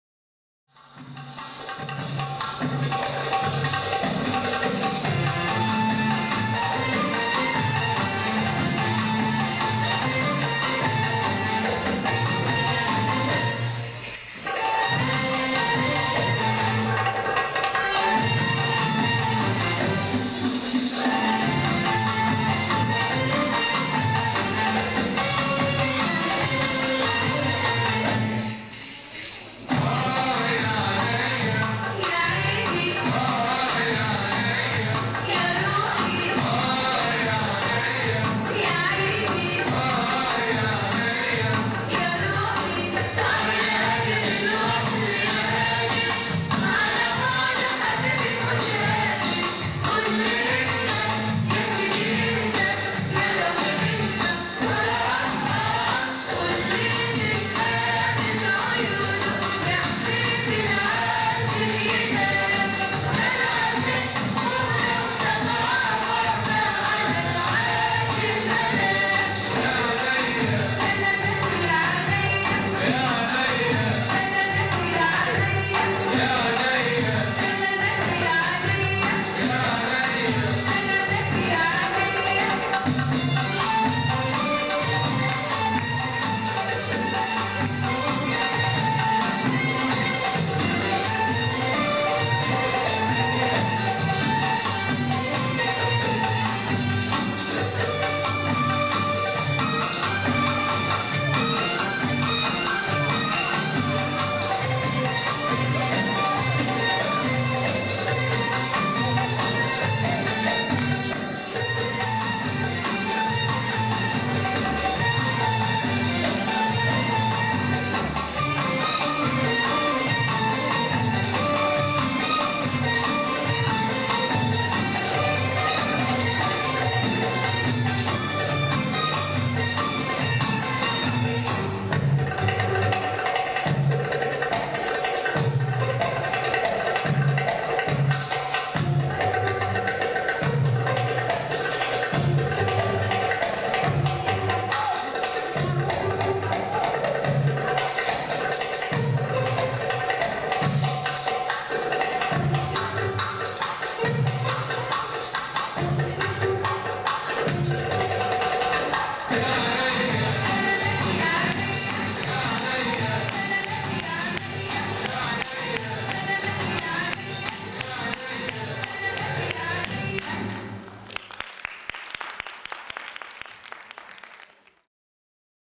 Olga Lachkova ⊰⊱ Ukrainian Cup '14.
Ольга Лачкова на Шестом открытом Кубке Украины исполнителей восточного танца в Городском Дворце детского и юношеского творчества г. Донецк 02.02.14.
(Синьоры кандидаты в профессионалы фольклор) 64,80 .